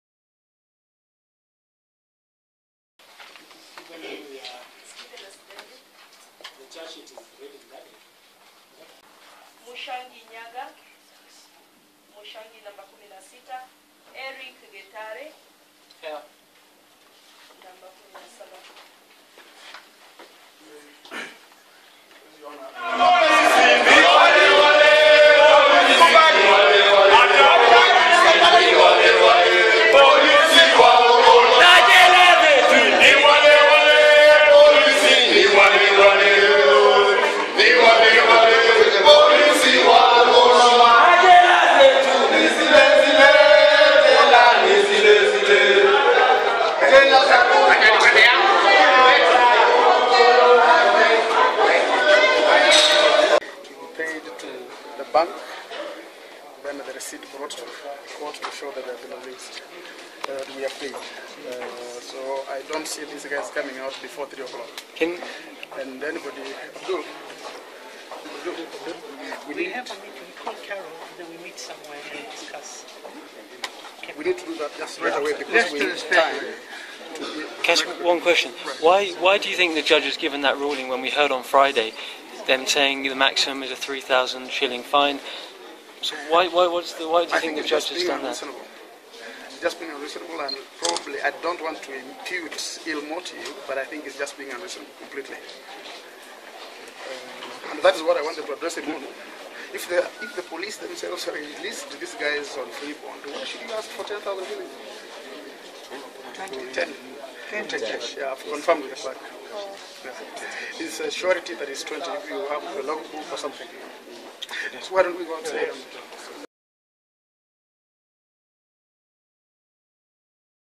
Arrested activists refuse to take plea
Seventeen protesters arrested during the anti-Mps pay demo in Nairobi recently refused to plead to criminal charges yesterday. They said the charges "lacked sufficient disclosure" and asked a magistrate to declare the case "unconstitutional" and set them free. However, they were ordered to deposit cash bails of Sh 10,000 each pending a ruling on the objection raised against the prosecution. "The constitution demands that anyone arrested should be informed of the charge, given sufficient details, understand it first before pleading," Mr Harun Ndumbi appearing for the activists said.